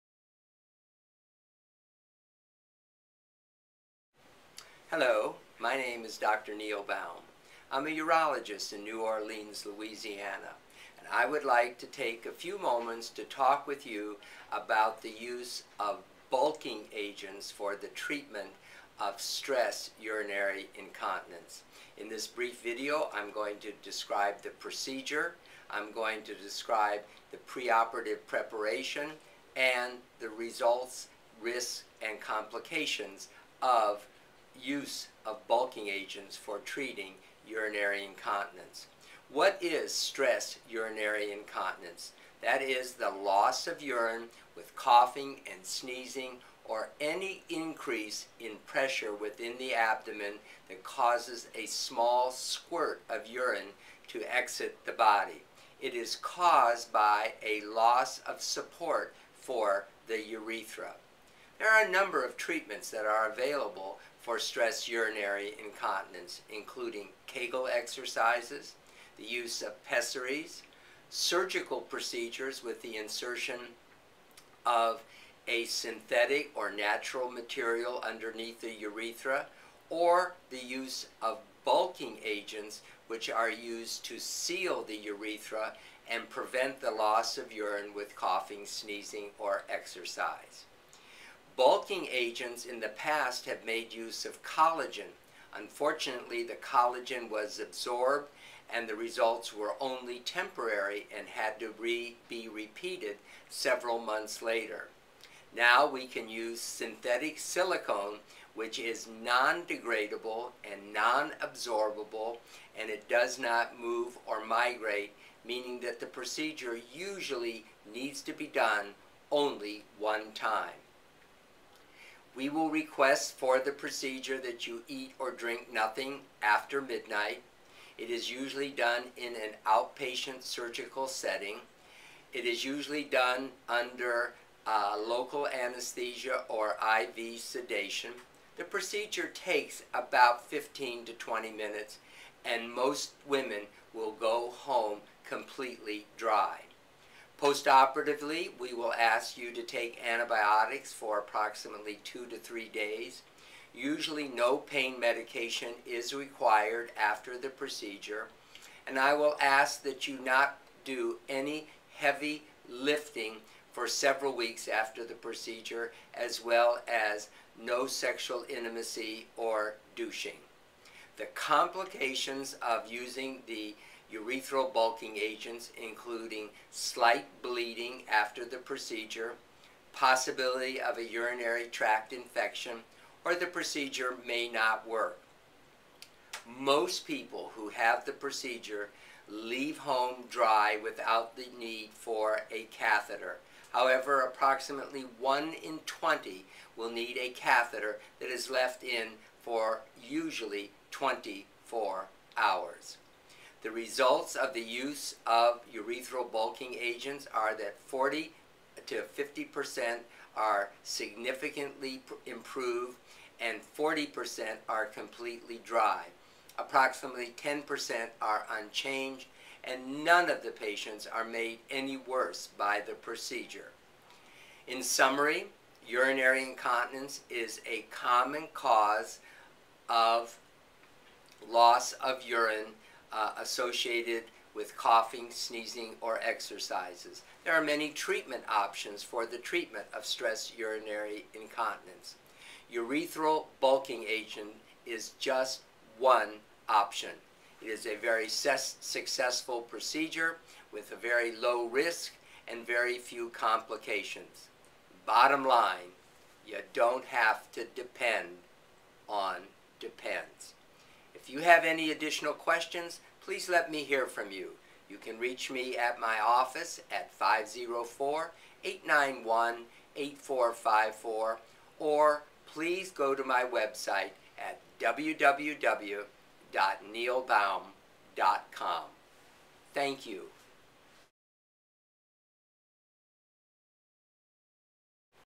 When It Gets Wet "Down There"-Use of Bulking Agents To Treat Urinary Incontinence
Stress incontinence is one of the most common causes loss of urine in women.  the condition is common after pregnancy and after menopause.  There are many treatment options including the use of bulking agents around the urethra, the tube from the bladder to the outside of the body.  The treatment takes 20 minutes and most men are dry after the procedure.  You don't have to depend on Depends.